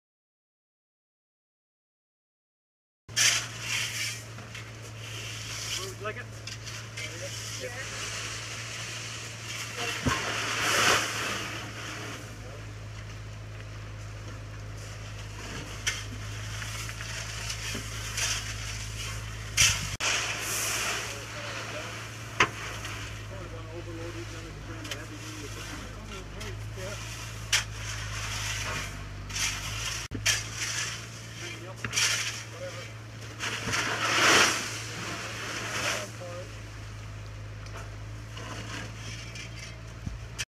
Moving gravel at Mossom Dec. 5.14
We are protecting the foundation of our new hatchery and education centre from frost-heave by backfilling with gravel .